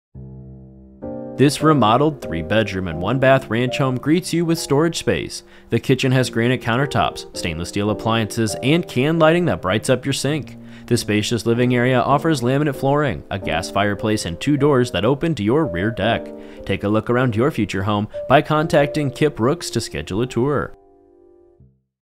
2834 Arrowsmith Drive Reynoldsburg, OH | ColdwellBankerHomes.com
2834 Arrowsmith Drive, Reynoldsburg, OH

Check out this completely remodeled three bedroom and one bath ranch style home. Enter through your front door that greets you with a storage space that can be used for shoes or other items you chose. The entry way and kitchen offers a neutral tile floor that brightens up the home. The newer kitchen